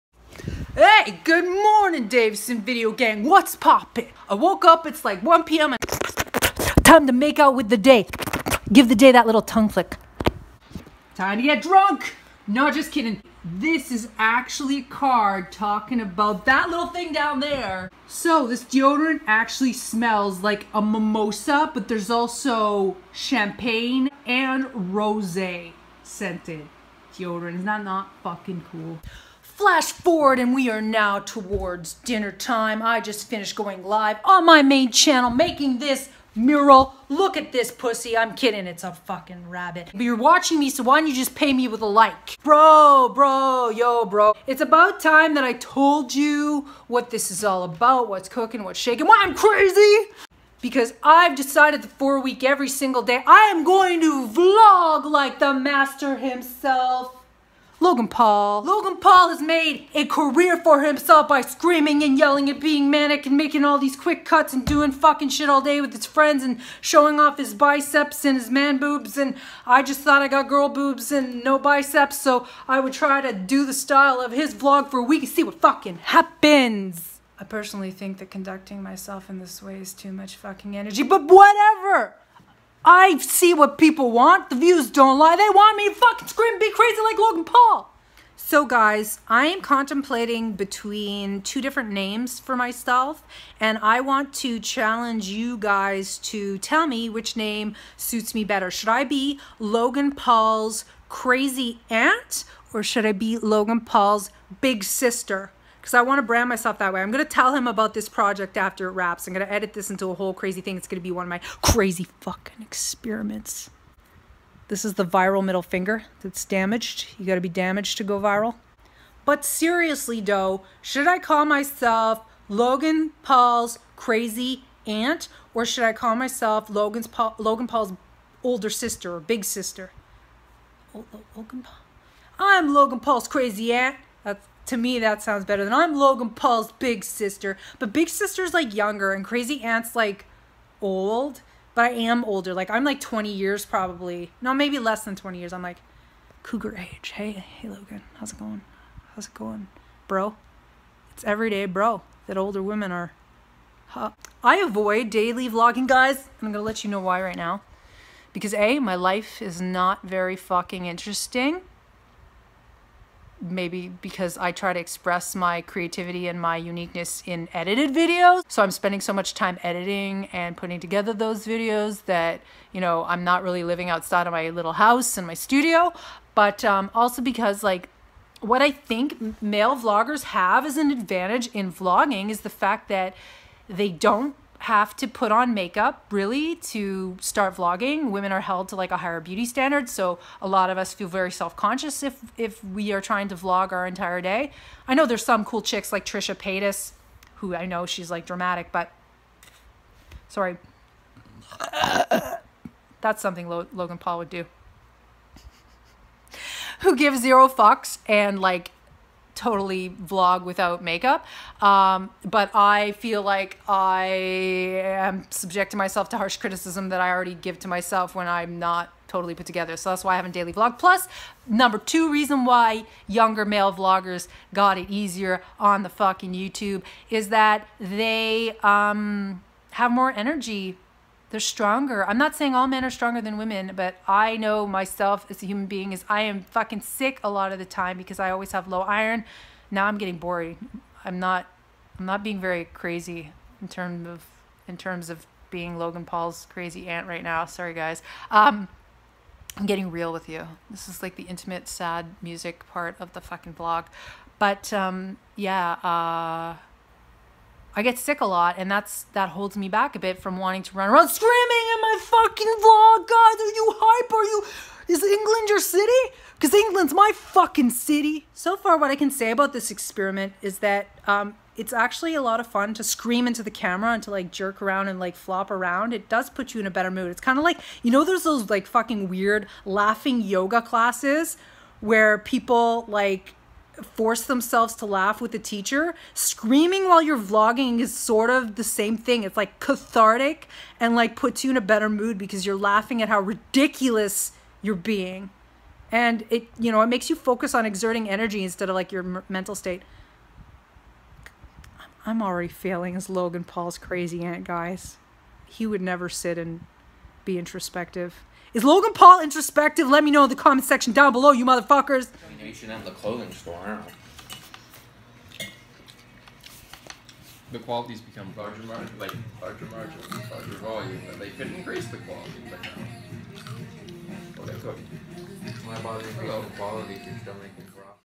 Logan Paul For A Week - Day 1
I am vlogging like Maverick superstar Logan Paul Vlogs for a week to experience what his video style is like and to see what happens or changes when I do. Will I become famous or just get let go of from my Disney contract? Let me know what you think of my experiment in the comment section!

I guess this is kind of a parody, too.